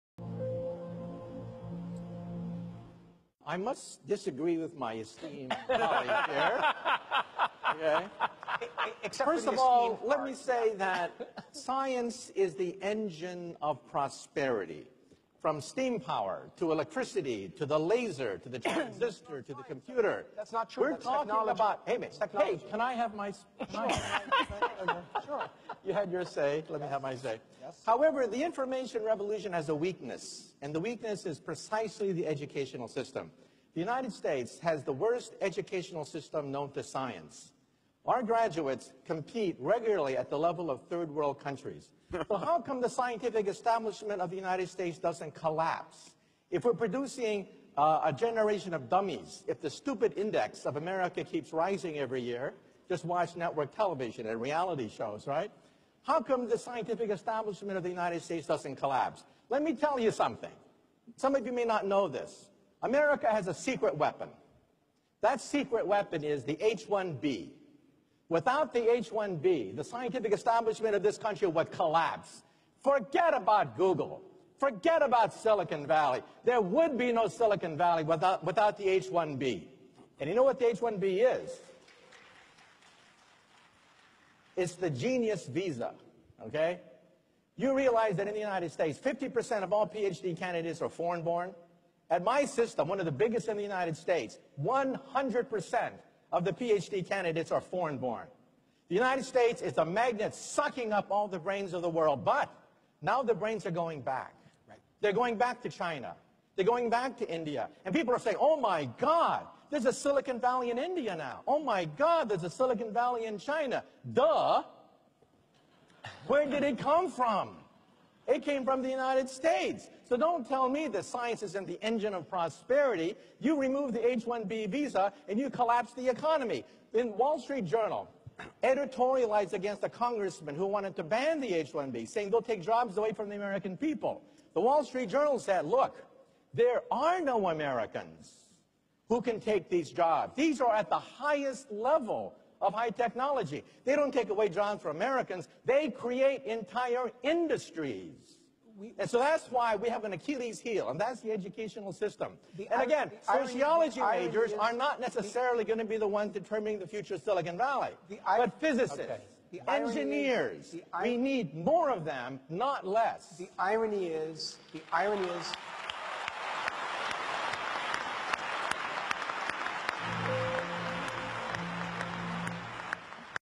حقيقة العلم والتعليم في أمريكا بدون مهاجرين - ميتشيو كاكو
من ترجمة صفحة : في أمريكا والدول المتقدمة - المقطع ينقل لنا موقفا نادرا عندما أغضبوا عالم الفيزياء النظرية اللاديني ميتشيو كاكو (من أبوين يابانيين) : فدفعوه لهدم الهالة المزيفة حول أمريكا والهزيمة النفسية لباقي دول العالم (ومنهم الملاحدة العرب) عندما يواجهك أحدهم بجهله قائلا بكل غباء : لا يوجد علماء مسلمين اليوم ؟! وبفضل الله نشرنا عشرات المنشورات عن علماء وباحثين مسلمين في بلادنا وفي أرقى الجامعات العالمية اليوم في كل المجالات